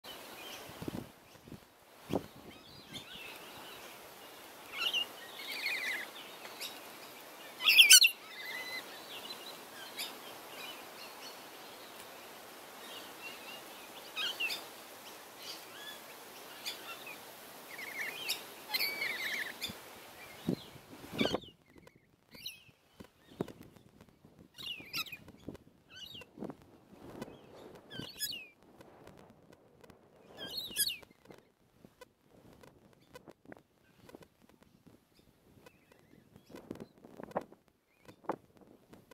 Rainbow Lorikeet in Red Flowering Gum
Rainbow Lorikeet using their tongue to gathering pollen and nectar from a red flowering gum.  Corymbia ficifolia, commonly known as the red flowering gum, is one of the most commonly planted ornamental eucalypts.   Flowering gums produce a spectacular display of colour in summer.